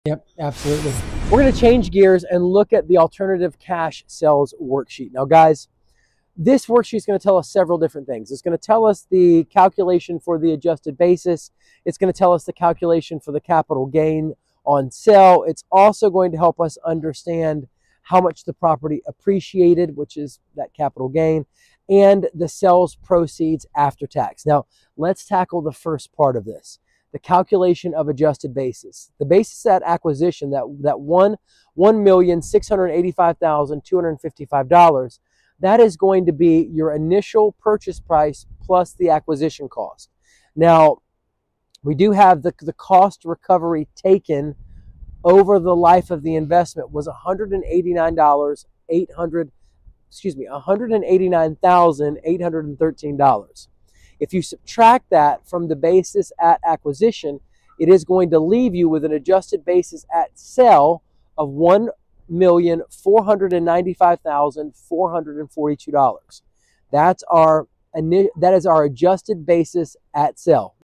Cash Sales Worksheet: Calculate Adjusted Basis & Capital Gain
We'll explore the alternative cash sales worksheet, detailing adjusted basis and capital gain calculations. We'll also understand property appreciation and after-tax sales proceeds. The initial purchase price, plus acquisition costs, minus cost recovery equals adjusted basis at sale.